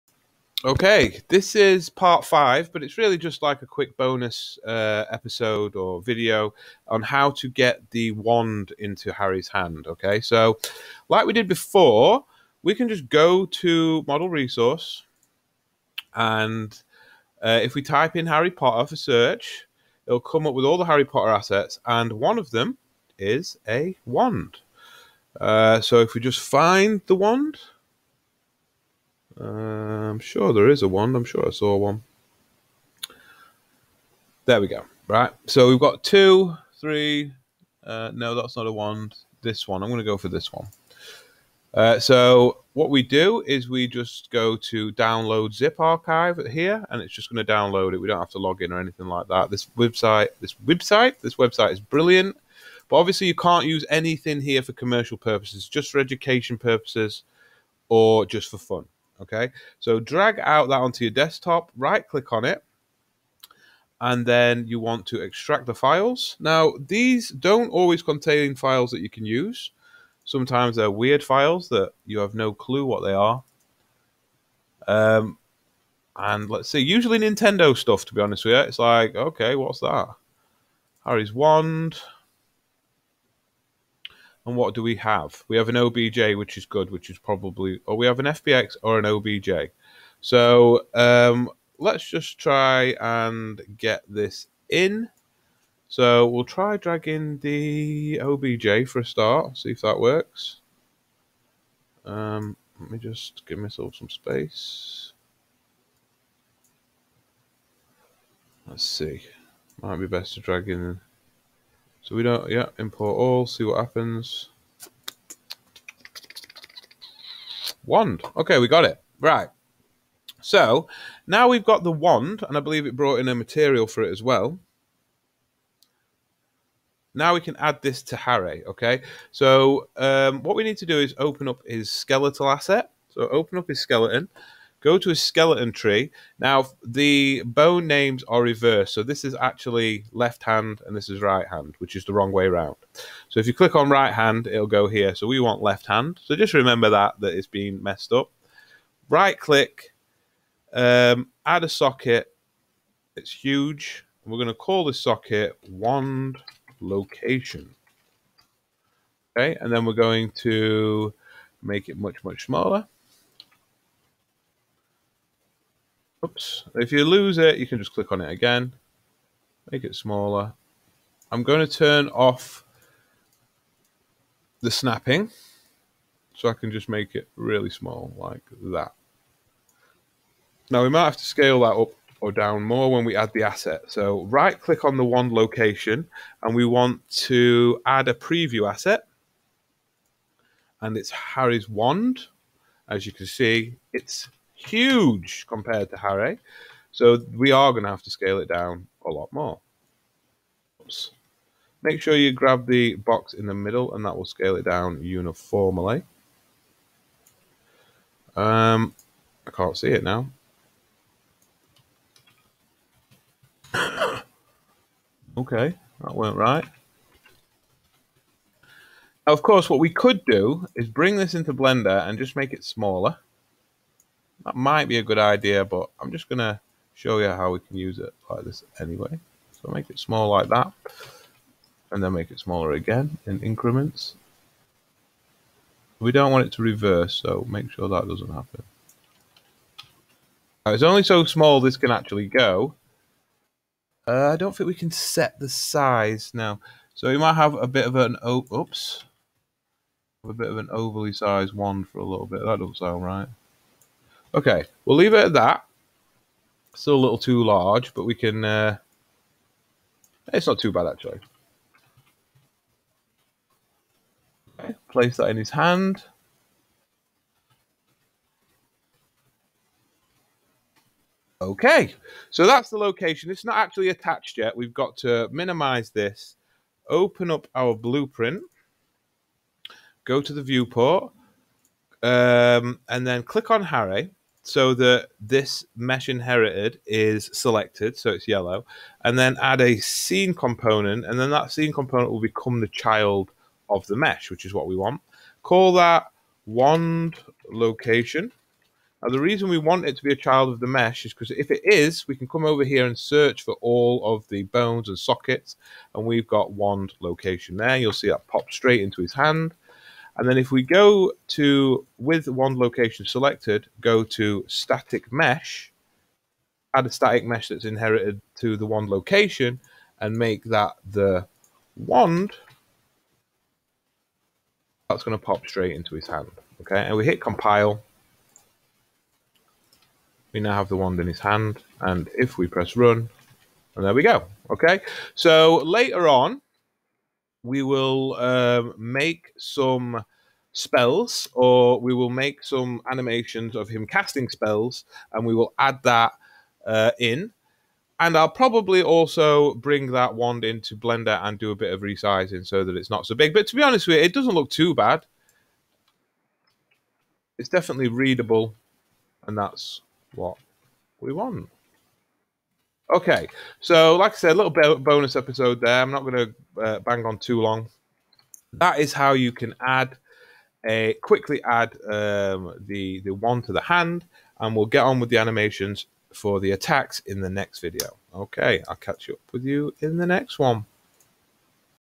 Lets Create: Harry Potter. Part 5. Adding the Wand
In this series we will create characters from Film, TV and games.
In the first set of videos we will be going over how to create and set up animations for a third person Harry Potter character.
Follow along step by step using Blender and Unreal Engine 4.